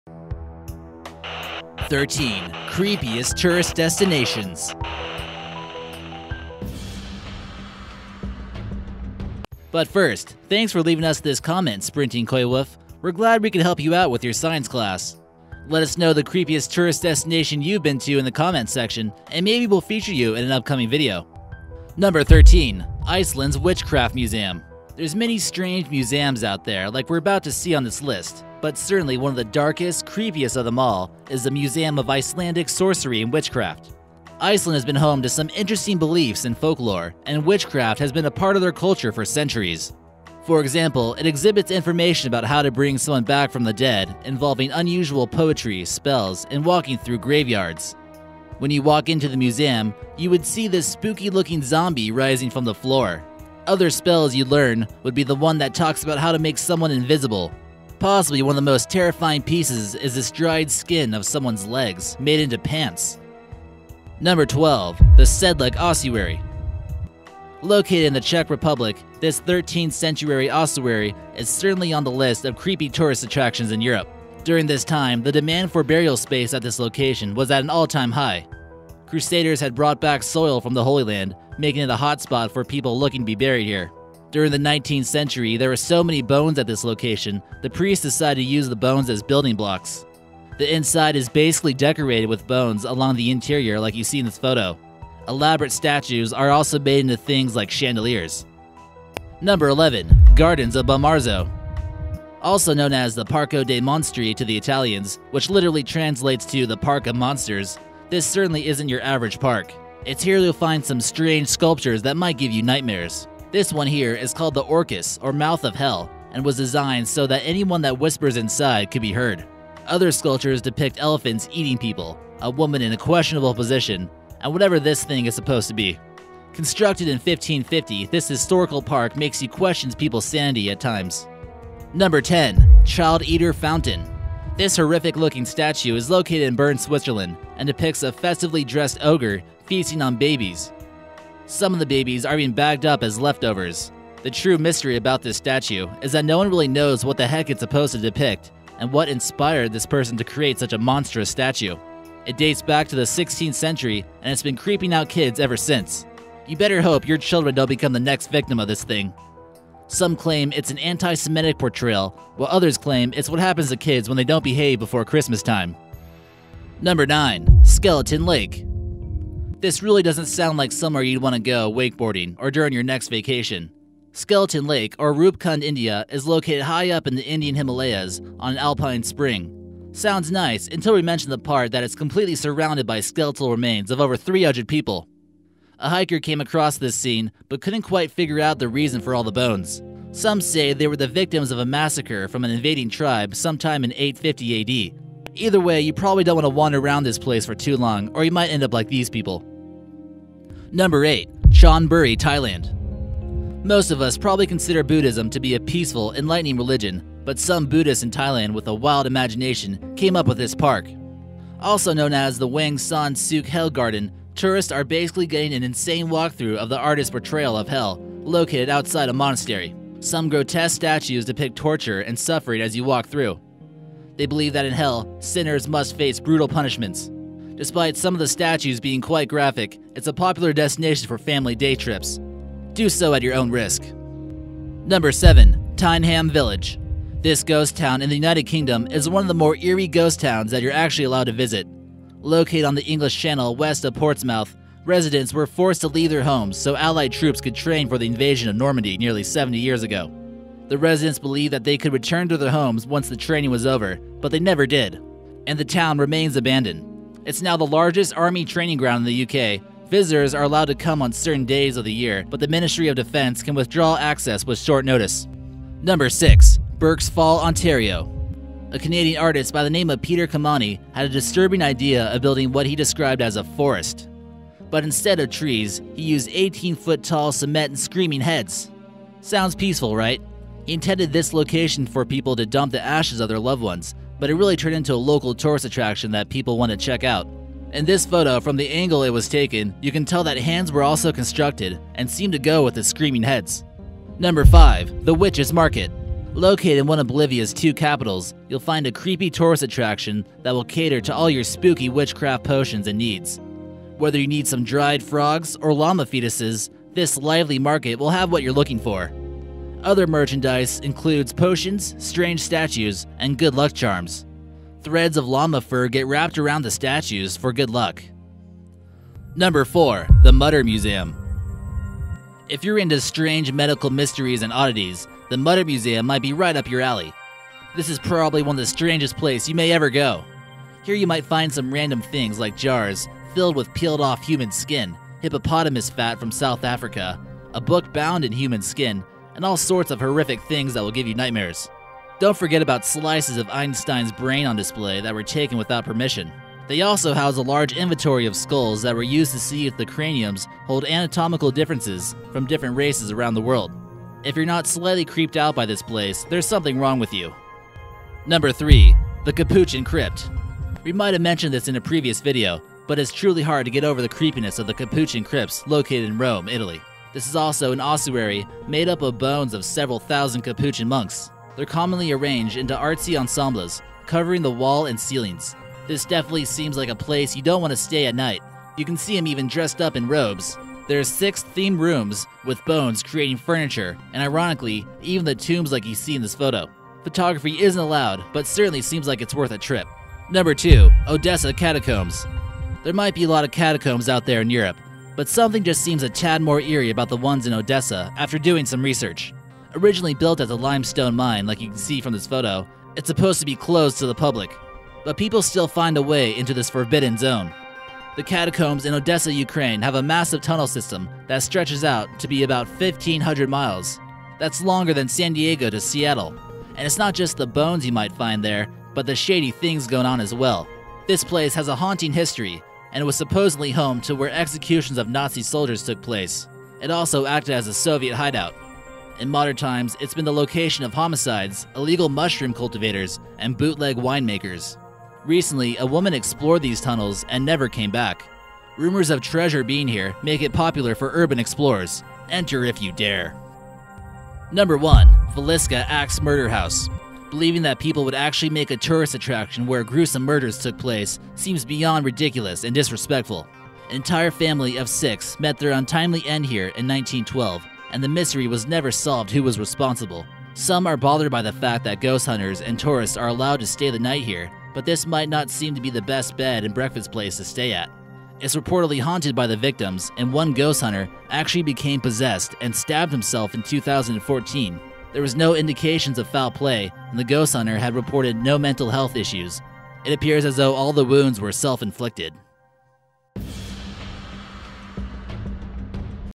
13 Creepiest Tourist Destinations
Visit these creepy but poplular tourist destinations & attractions all over the world from catacombs to a spooky museum of witchcraft!

7. Tyneham Village
This ghost town in the United Kingdom is one of the more eerie ghost towns that you’re actually allowed to visit. Located on the English Channel, just west of Portsmouth, residents were forced to leave their homes so allied troops could train for the invasion of Normandy nearly 70 years ago. The residents believed that they could return to their homes once the training was over but they never did. It was deemed that the land was too strategic to the military and the town remains abandoned. It’s now the largest army training ground in the UK. visitors are allowed to come on certain days of the year but the Ministry of Defense can withdraw access with short notice.

6. Burk’s Fall Ontario
A Canadian artist by the name of Peter Camani had a disturbing idea of building what he described as a “forest”. But instead of trees, he used 18 foot tall cement screaming heads! Sounds peaceful right? He intends this location for people to dump the ashes of their loved ones but it’s really turned into a local tourist attraction that people want to check out for their own. In this photograph, from the angle it was taken, you can tell that hands were also constructed and seem to go with the screaming heads.

5. The Witches Market
Located in one of Bolivia’s two capitals, you’ll find a creepy tourist attraction that will cater to all your spooky witchcraft potions and needs. Whether you need some dried frogs, or llama fetuses, this lively market will have what you're looking for. Other merchandise includes potions, strange statues, good luck charms. Threads of llama fur get wrapped around the statues for added good luck.

4. The Mutter Museum
If you’re into strange medical mysteries and oddities, the mutter museum might be right up your alley. This probably one of the strangest places you may ever go! Here you might find random things like jars filled with peeled off human skin, hippopotamus fat from South Africa, a book bound in human skin and all sorts of horrific things that will give you nightmares! Don’t forget about the slices of Einstein’s brain on display that were taken without permission! They also house a large inventory of skulls that were used to see if the craniums hold anatomical differences vary from different races around the world. If you’re not slightly creeped out by this place, there’s something wrong with you.

3. Capuchin Crypt
We might have mentioned this in a previous video, but it’s truly hard to get over the creepiness of the Capuchin Crypts located in Rome, Italy. This is also an ossuary and made up of the bones of several thousand Capuchin monks. They’re commonly arranged in into artsy ensembles, covering the walls and ceilings. This definitely seems like a place you don’t want to stay the night! Like you see in this photo, you can see them dressed up in robes with traditional brown robes! There’s about 5 or 6 themed rooms with bones creating furniture and ironically, even tombs like you see in this photo. Photography isn’t allowed but certainly seems like it’s worth a trip!

2. Odessa Catacombs
There might be a lot of catacombs out there in Europe, but something just seems a tad more eerie about the ones in Odessa after doing research. Originally built as a limestone mine like you can tell from this photo, it’s supposed to be closed to the public, but people still find away into this forbidden zone. The catacombs in Odessa, Ukraine have a massive tunnel system that stretches out to be 1500 miles under this slavic beach town. That’s longer than San Diego to Seattle! And it’s not just from the bones you might find there, but the shady things going on, as well as its haunting history. It’s supposedly here where executions of Nazi soldiers took place and also acted as a soviet hideout. In modern times, it’s been the location of homicides, illegal mushroom cultivators and bootleg winemakers. Recently a woman explored these tunnels and never came back  Rumors of treasure being here as made urban explorers. Enter if you dare.

1.Villisca Axe Murder House
Believing that people would actually make a tourist attraction where gruesome murders took place seems beyond ridiculous and disrespectful. An entire family of 6 met their untimely end here in 1912 and the mystery was never solved who was responsible. Some are bothered by the fact that ghost hunters and tourists are allowed to stay the night here but this might not seem to be the best bed and breakfast place to stay at. It’s reportedly haunted by the victims and one ghost hunter actually became possessed and stabbed himself in 2014 during a routine investigation. There were no indications of foul play, the ghost hunter had no reported mental health issues and the wounds were self inflicted